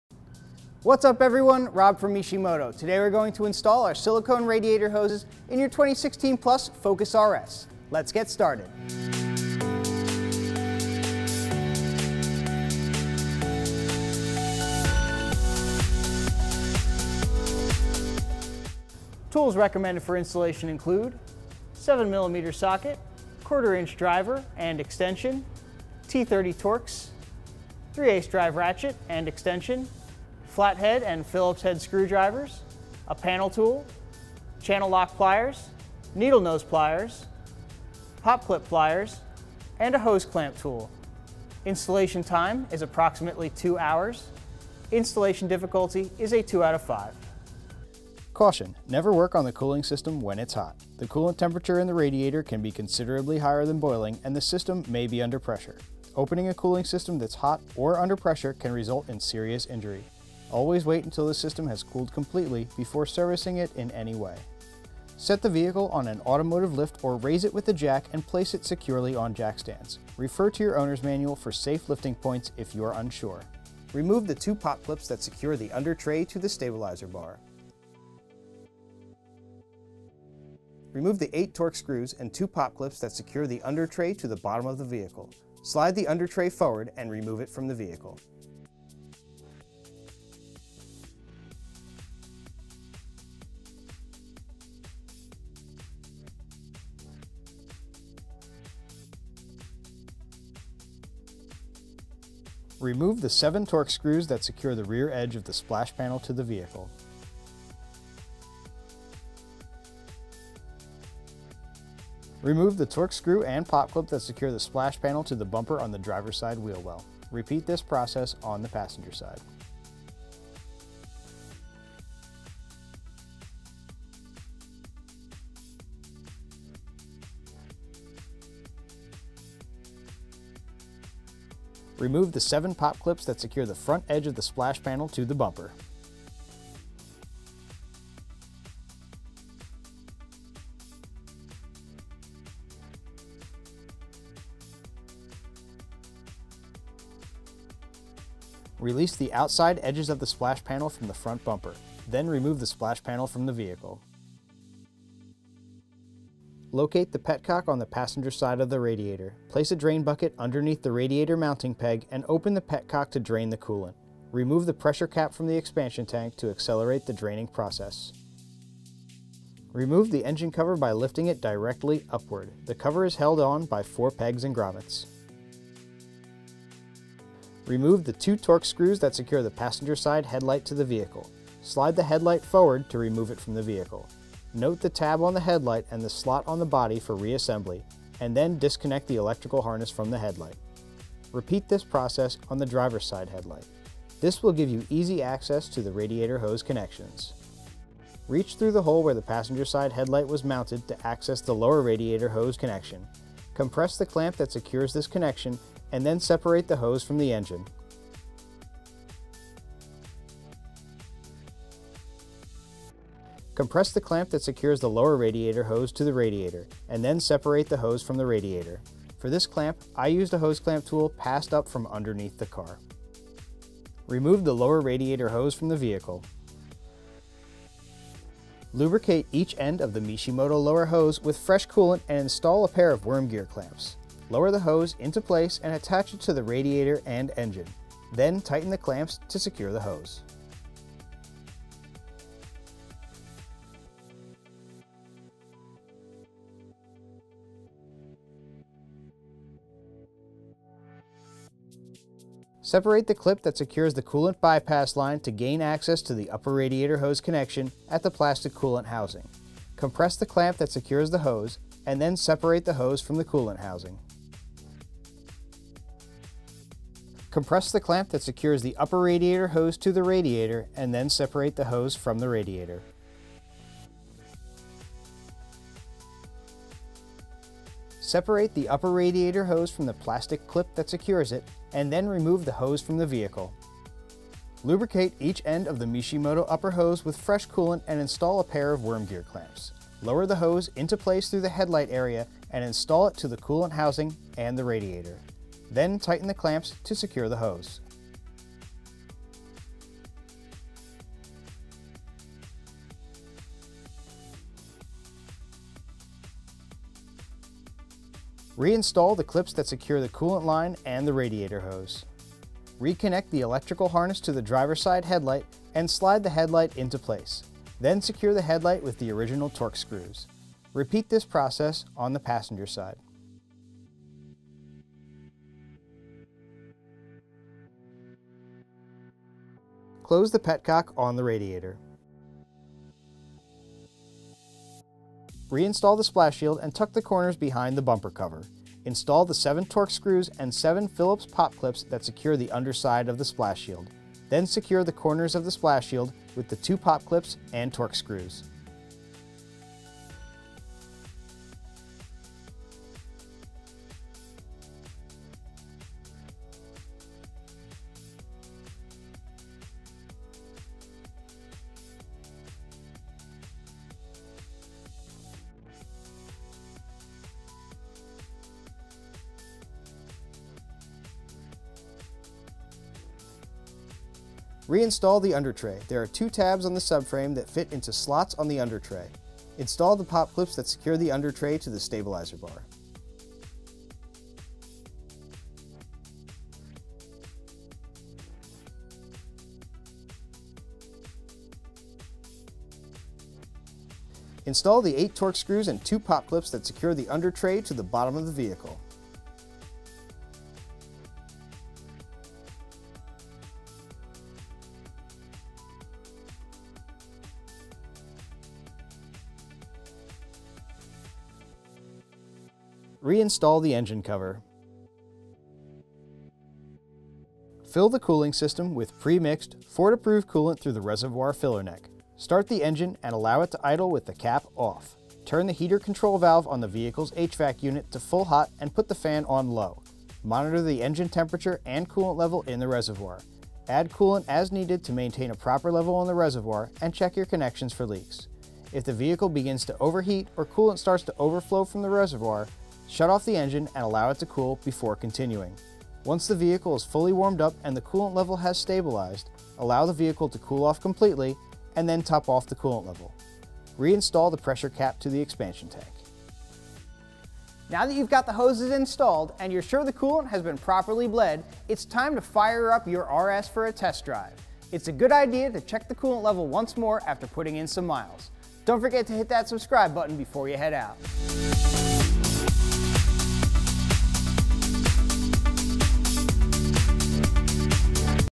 2016-2018 Ford Focus RS Silicone Radiator Hoses Installation Guide by Mishimoto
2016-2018 Ford Focus RS Silicone Radiator Hoses

MODEL: MMGP-RS-16
•Direct fit for the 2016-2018 Ford Focus RS
•Includes upper and lower radiator hoses
•Max temperature of 330°F
•Constructed from four layers of silicone with heat-resistant embedded fibers
•Increases heat and pressure tolerance
•Includes high-quality worm gear clamps for easy installation
•Available in black, red and Nitrous Blue

MP products.